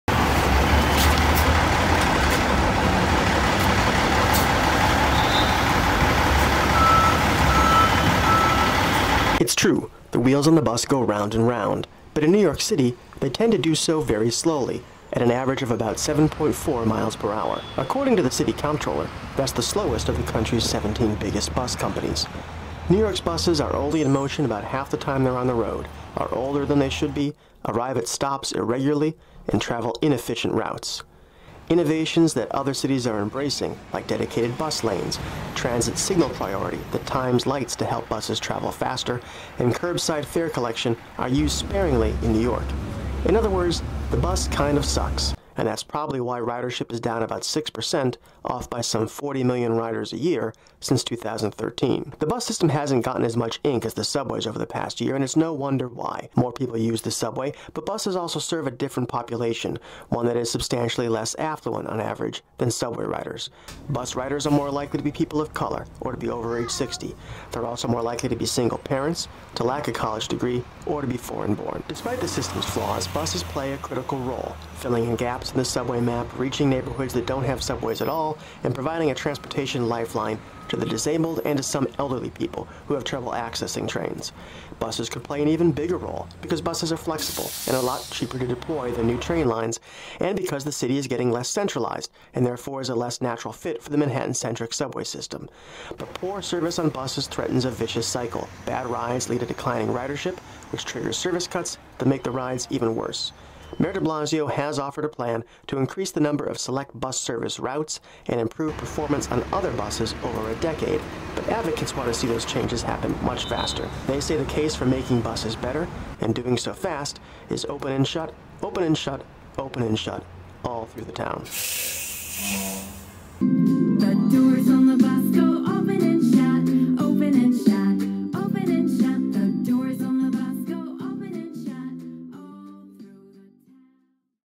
Your 2-Minute Briefing on: NYC's Bus System
What's wrong with the city's buses? What's right with them? A video by Marc Bussanich with Jarrett Murphy.